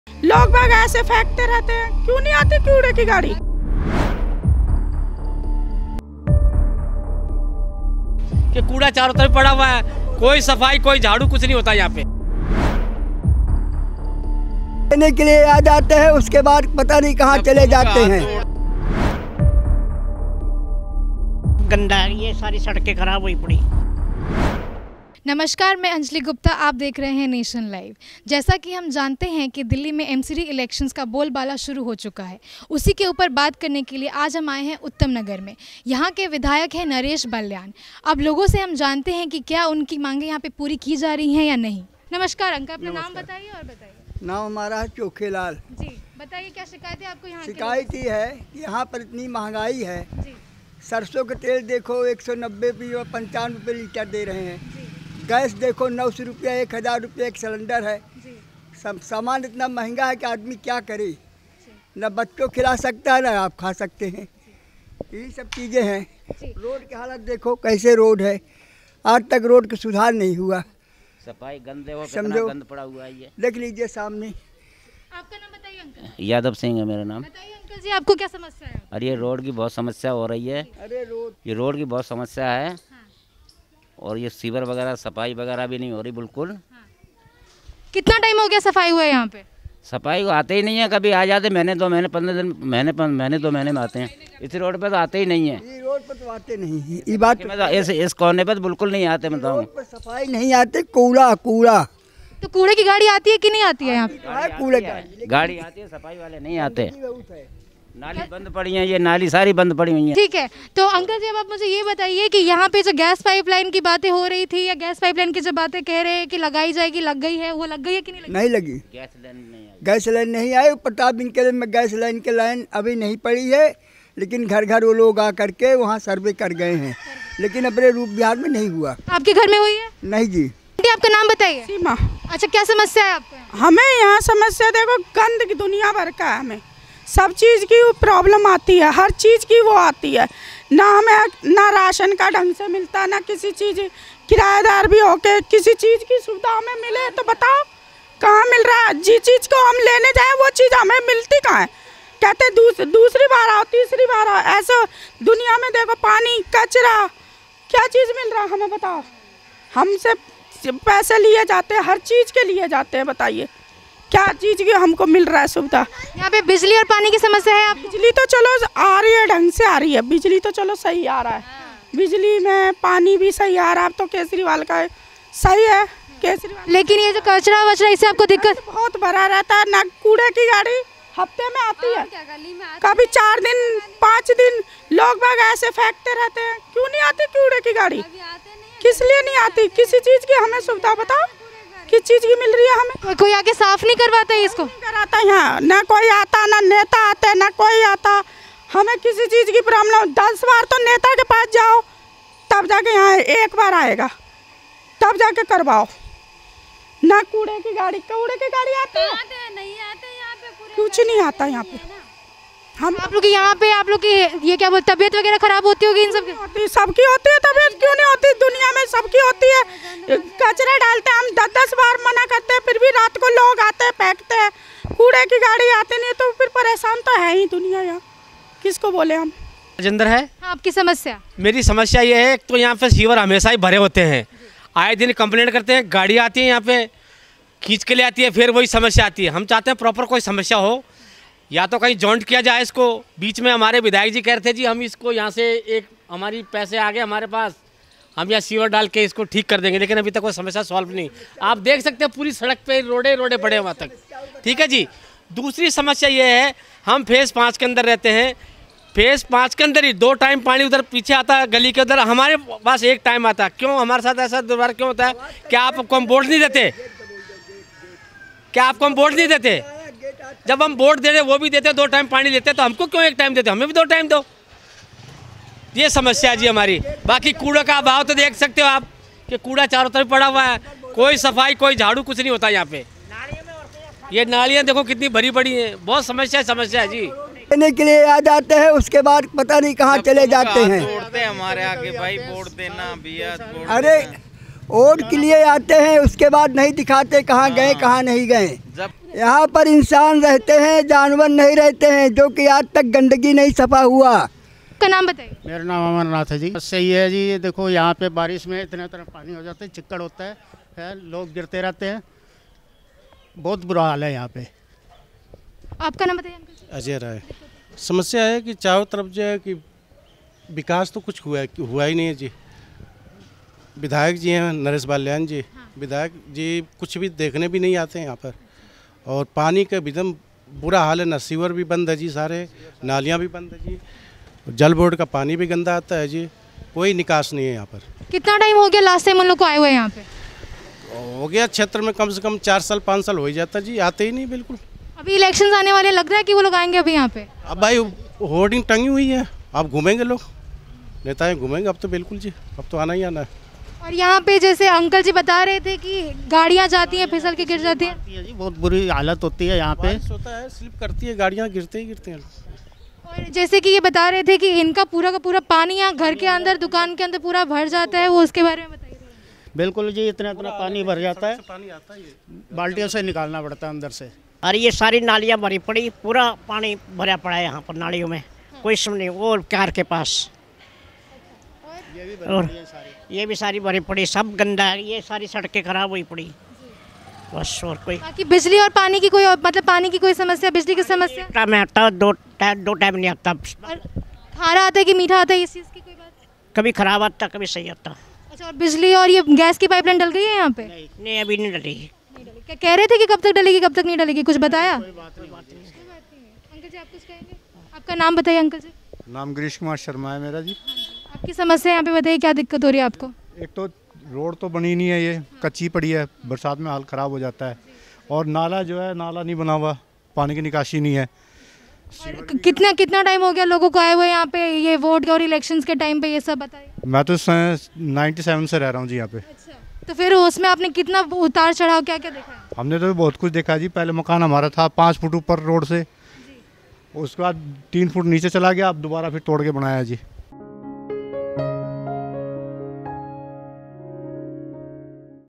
MCD Elections Delhi । Public Opinion । Uttam Nagar । निगम और दिल्ली सरकार की बदहाली बताती जनता
Nation Live iptv , नेशन लाइव आई पी टीवी

MCD Elections Delhi । Public Opinion । Uttam Nagar । निगम और दिल्ली सरकार की बदहाली बताती जनता | Nation Live | Nigam Chunaav |